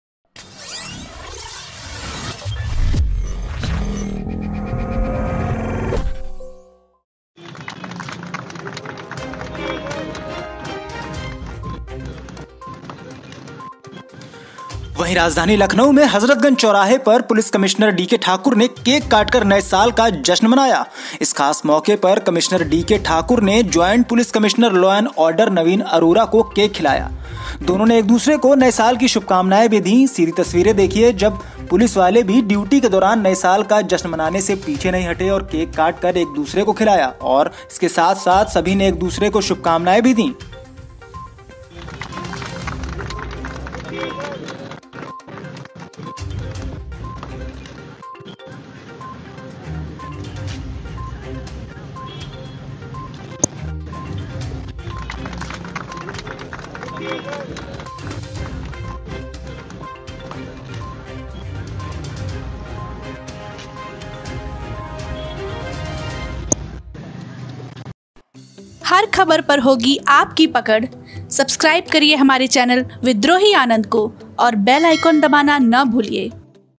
New Year 2021 : Lucknow Police ने Cake काटकर ऐसे मनाया जश्न
राजधानी लखनऊ में हजरतगंज चौराहे पर पुलिस कमिश्नर डी.के ठाकुर ने केक काटकर नए साल का जश्न मनाया इस ख़ास मौके पर कमिश्नर डी.के ठाकुर ने जॉइंट पुलिस कमिश्नर (लॉ एंड ऑर्डर) नवीन अरोरा को केक खिलाया दोनों ने एक दूसरे को नए साल की शुभकामनाएं भी दीं सीधी तस्वीरें देखिए जब पुलिस वाले भी ड्यूटी के दौरान नए साल का जश्न मनाने से पीछे नहीं हटे और केक काटकर एक दूसरे को खिलाया गया और शुभकामनएं दीं